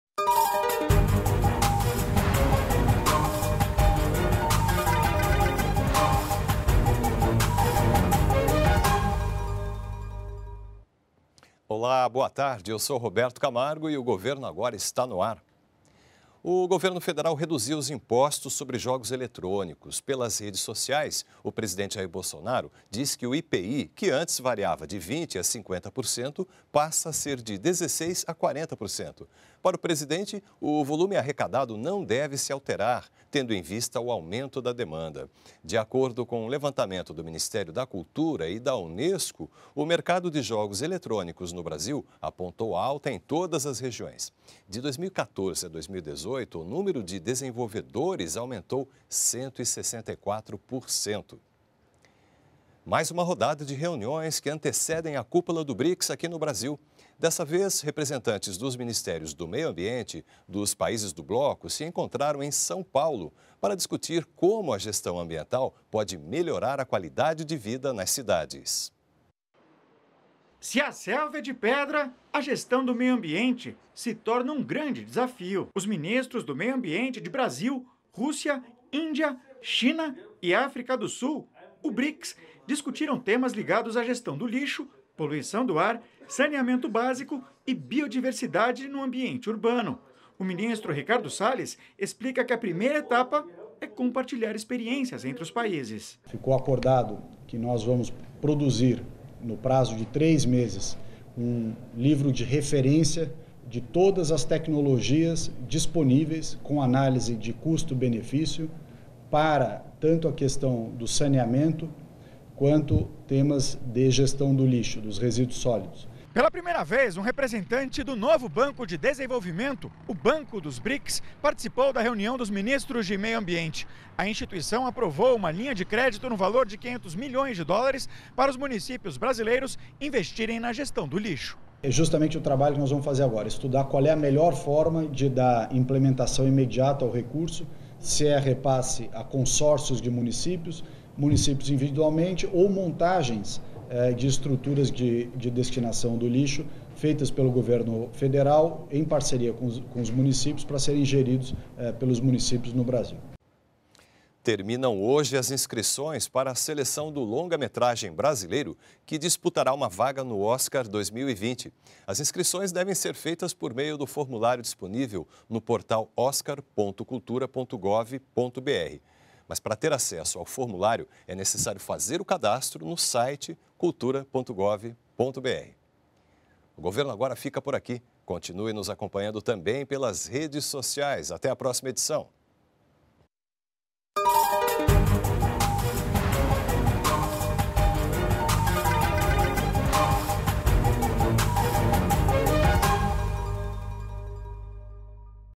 Governo Federal reduziu impostos sobre Jogos Eletrônicos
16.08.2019 - O Governo federal Reduziu os Impostos sobre Jogos Eletrônicos. Representantes de Ministérios do Meio Ambiente dos países do BRICS se reuniram em São Paulo. Terminam hoje as inscrições para a seleção do Longa Metragem brasileiro que disputará uma vaga ao Oscar 2020.